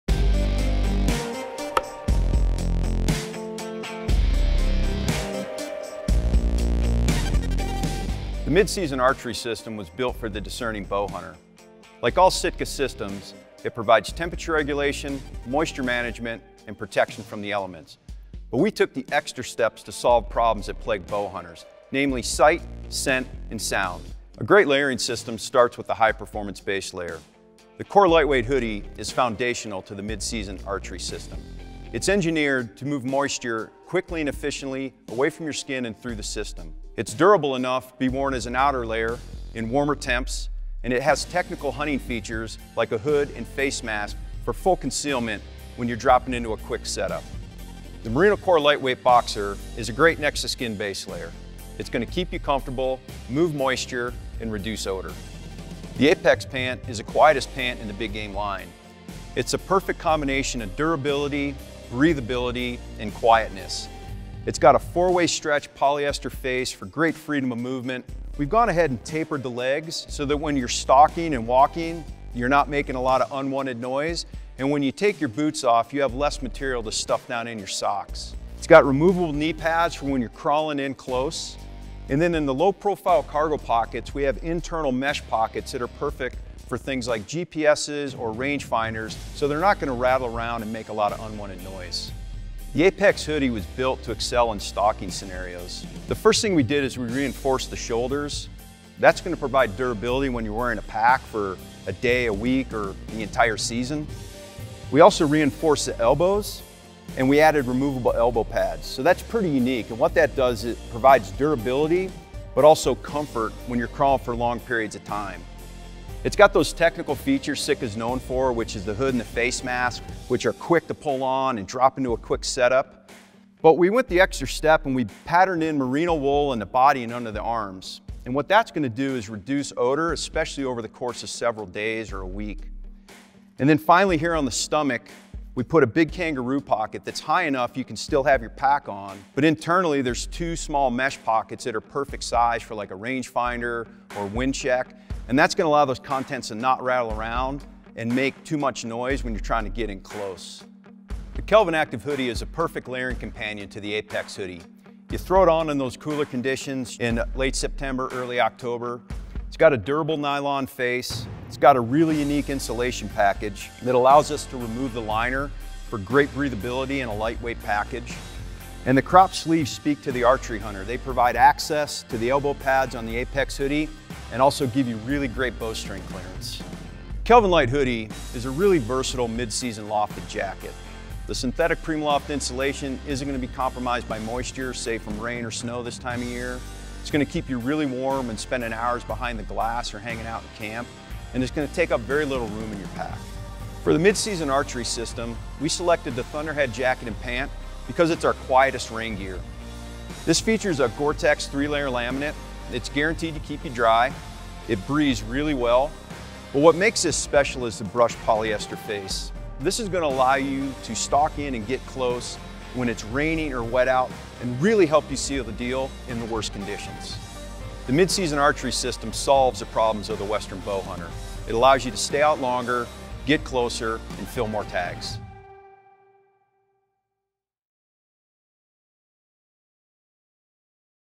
SITKA Systems: Mid-Season | Archery Big Game
The Mid-Season Archery System is designed to deliver comfort, performance and protection in the variable conditions experienced below tree-line during mid- to late fall while meeting the requirements of scent and sound suppression during close-quarter engagements.

This complete skin-to-shell system delivers modularity: layer up to beat the cold or shed layers during the heat of the afternoon and high-output climbs. The innovative fabrics and technologies used throughout are built to accelerate the movement of moisture away from the skin and through the system–while keeping the elements out–so you remain dry and comfortable. Reducing the chance a stalk is blown by sound or scent, outerwear layers are designed to be as quiet as possible while still providing protection from the elements; and blended synthetic and merino garments combat odor.

For optimum stealth, GORE™ OPTIFADE™ Concealment Subalpine technology is used across this entire system. Scientifically engineered to confuse the vision of antelope, elk, mule deer and other ungulates, it offers maximum concealment when stalking in vegetated terrain.